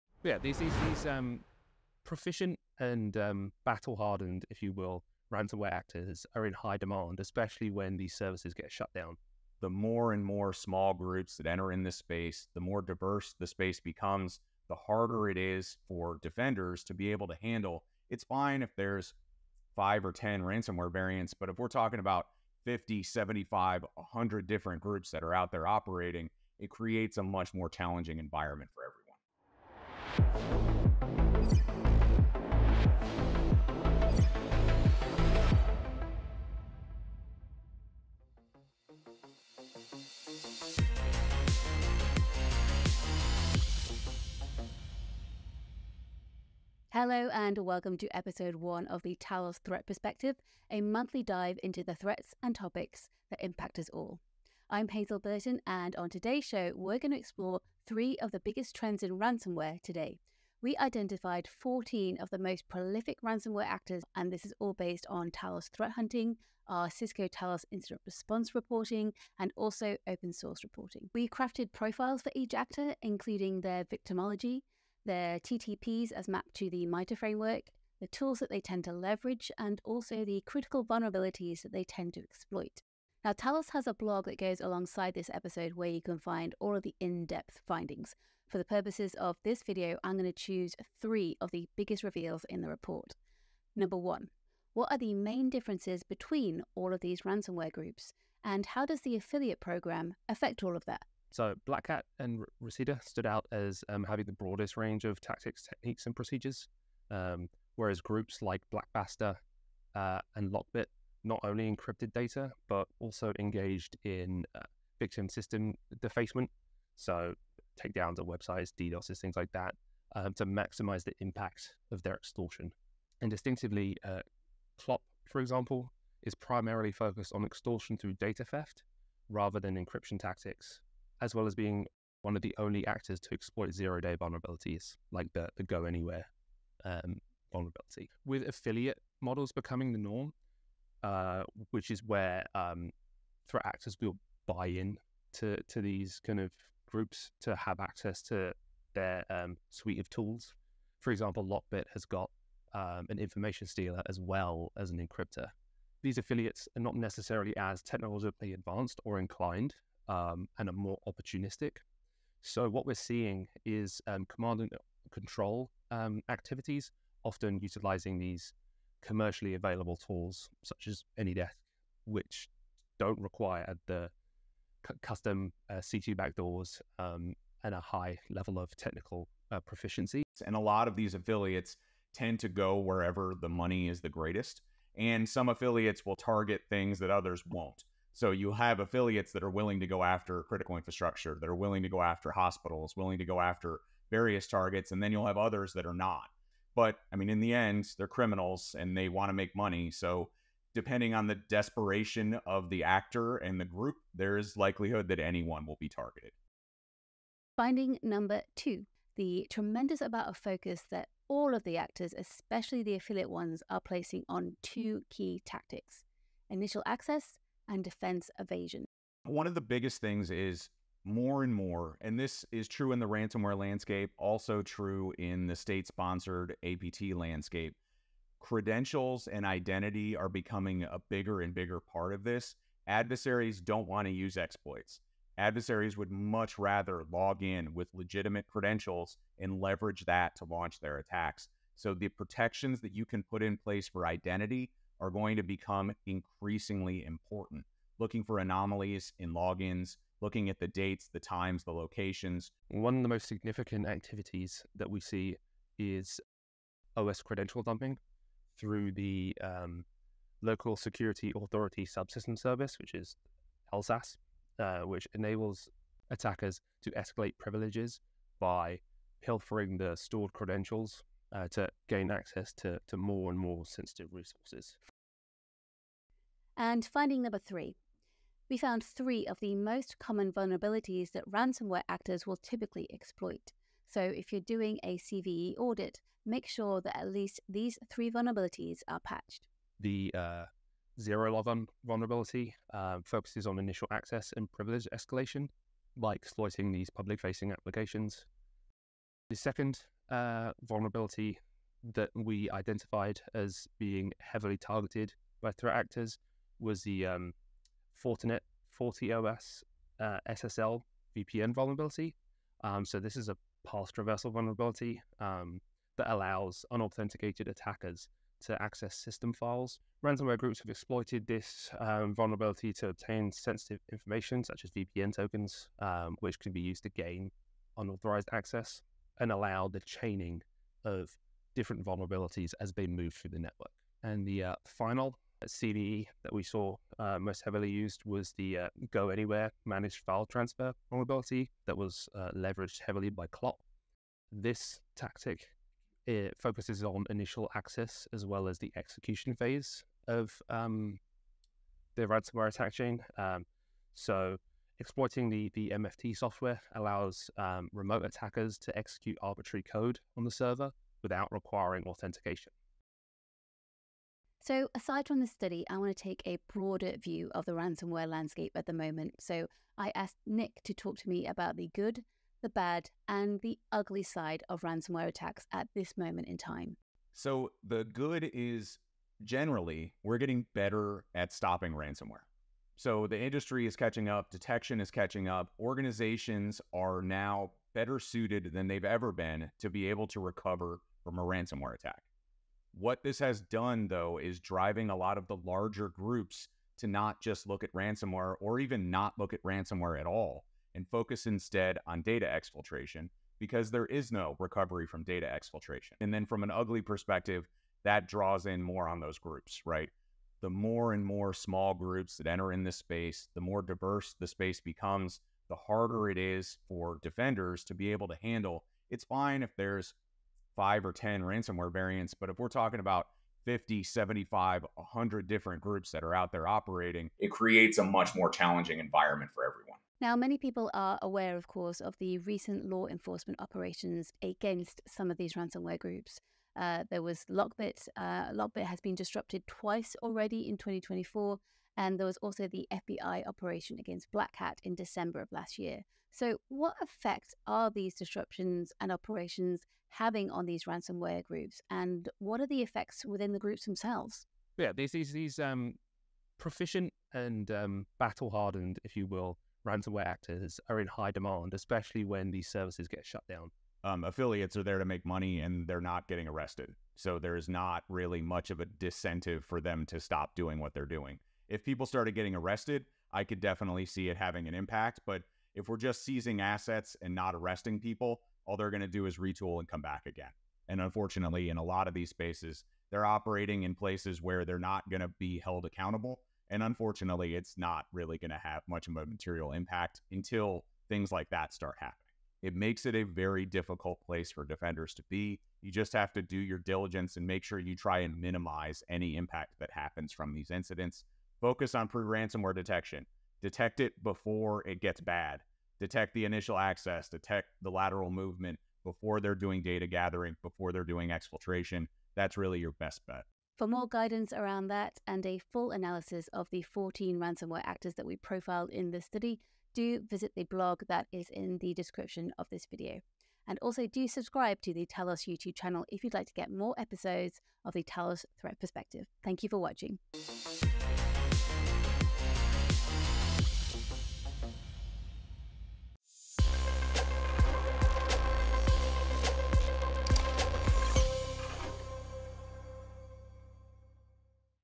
The TTP Episode 1: Inside the ransomware playbook
In the first of our new series, we explore new research on the most prominent ransomware groups. We pick 3 key trends to focus on: initial access, differences between the groups, and the vulnerabilities they most heavily target. Guests include Talos experts Nick Biasini and James Nutland.